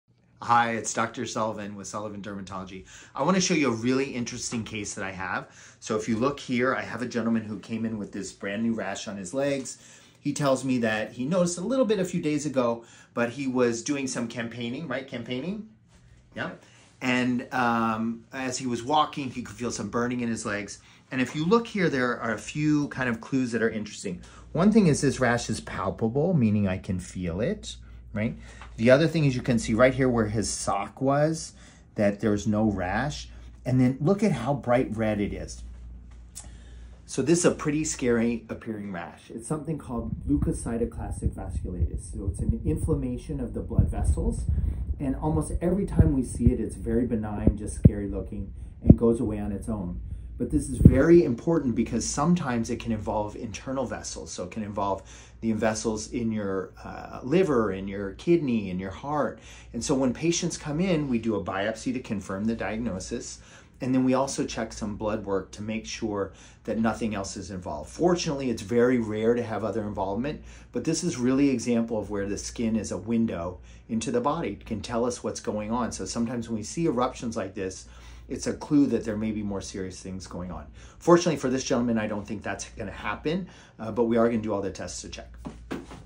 Vasculitis
Hi Dr. Tory Sullivan with Sullivan Dermatology, and today I want to share a fascinating case that really highlights how the skin can be a window into the body. This gentleman came into my office with a brand-new rash on his legs that he noticed just a few days ago. He mentioned that he was out campaigning and while he was walking, he started feeling a burning sensation in his legs. At first, he didn’t think much of it, but as the days went on, this rash developed.

Now, if you take a closer look at this case, there are a few interesting clues. First, this rash is palpable, meaning I can actually feel the raised areas of inflammation. That’s something that always draws my attention because not all rashes have this characteristic. Another clue is that there’s a clear line where his sock was—no rash in that area. It’s almost as if the rash spared that section of the skin, which tells me this could be something related to how the blood vessels are reacting. Lastly, the rash itself is very bright red, which can be pretty alarming at first glance.

What we’re seeing here is a condition called leukocytoclastic vasculitis. This is an inflammation of the small blood vessels in the skin. While it looks scary and can cause discomfort, the good news is that in the majority of cases like this, it’s benign and tends to go away on its own without causing long-term harm. However, it’s crucial to remember that vasculitis can sometimes involve internal organs. In rare cases, it can affect blood vessels in places like the liver, kidneys, or heart.

Because of that possibility, when a patient presents with this kind of vasculitis, we don’t take any chances. The first step is to perform a skin biopsy to confirm the diagnosis and rule out other conditions. In addition to that, we’ll also run blood tests to check for any signs that internal blood vessels might be involved. While it’s rare for this condition to extend beyond the skin, it’s something we need to be thorough about.

This case really illustrates how the skin can give us important clues about what might be happening inside the body. Sometimes a rash is more than just a skin issue—it can be a sign of underlying inflammation or other systemic conditions. Fortunately, for this patient, I don’t expect any further complications, but we’re going to do all the necessary tests to make sure we’ve ruled everything out and to provide him with the best care.

This is a great reminder of why it's important to pay attention to changes in your skin and consult a dermatologist if something doesn’t seem right. The skin can tell us so much about our overall health, and even though this rash might look intimidating, with proper diagnosis and testing, we can be confident in treating it effectively.